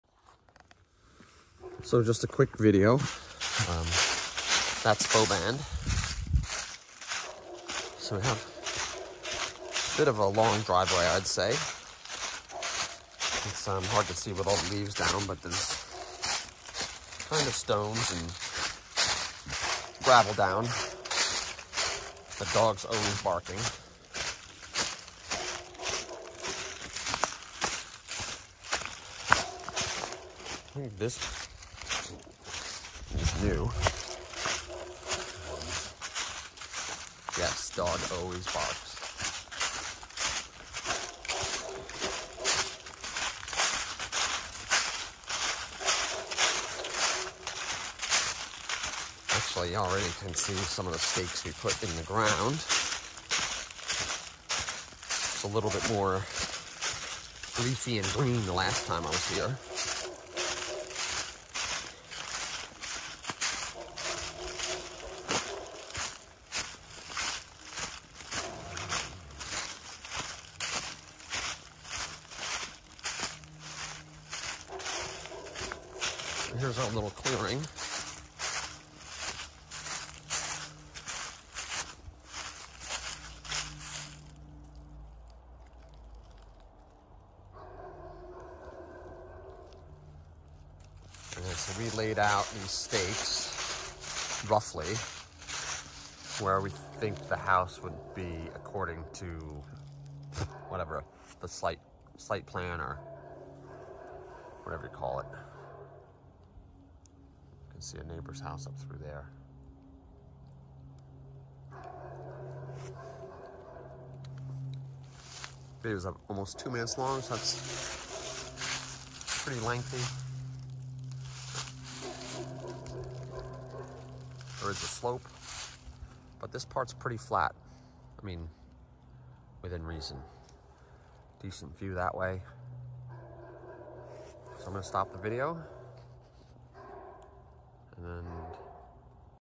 231 Boband Road Youngsville
This is a quick walk down from Boband to our land...